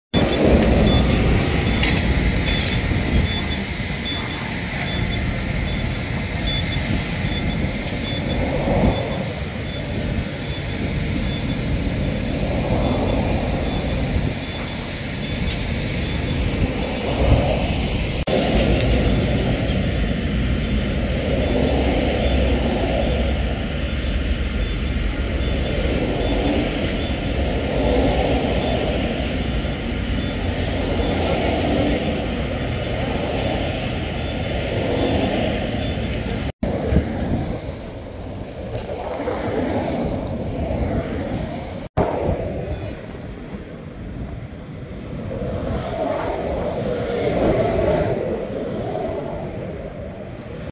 Incendio auto autostrada Pontecagnano
Le immagini della Mercedes distrutta dalle fiamme mentre percorreva l'autostrada Salerno Reggio Calabria. Il conducente è riuscito a mettersi in salvo.